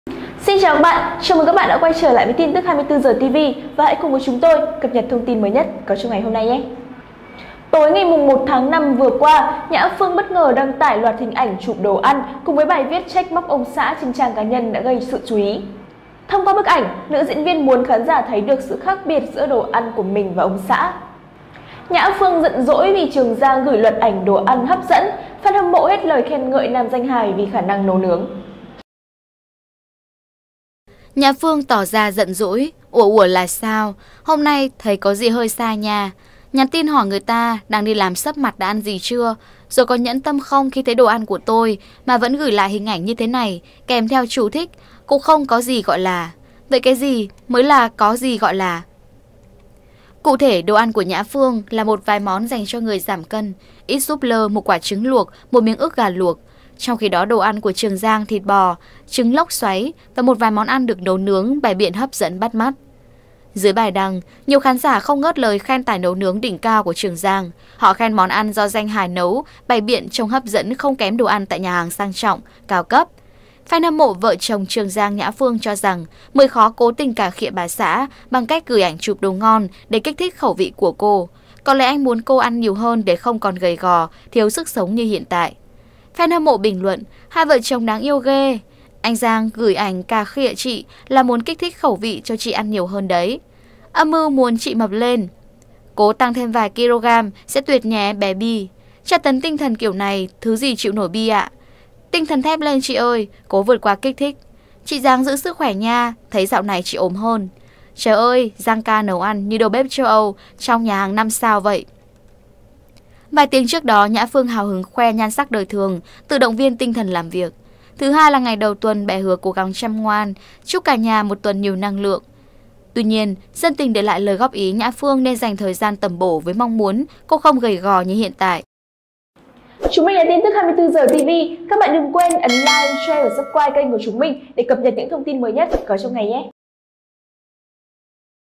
Nhã Phương Bất Ngờ Tra'ch Mo'c Ông Xã Nhưng CĐM Lại Khen Ngợi Trường Giang - TIN TỨC 24H TV
Nhã Phương Bất Ngờ Tra'ch Mo'c Ông Xã Nhưng CĐM Lại Khen Ngợi Trường Giang